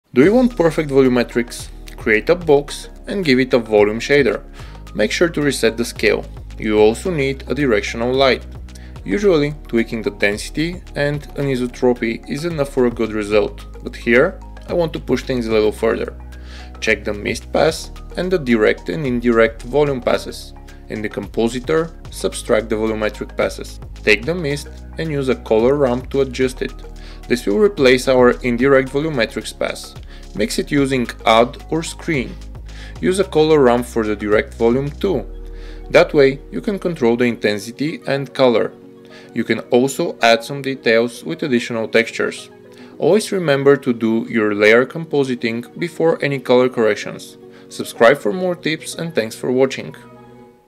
How to Composite Volumetric Lighting in Blender
A short tutorial on how to have better control over your volumetric lighting in Blender using Compositing
PC Specs
CPU : Intel Core i7 5820k
RAM : 32GB DDR4
GPU : ASUS TUF RTX 3090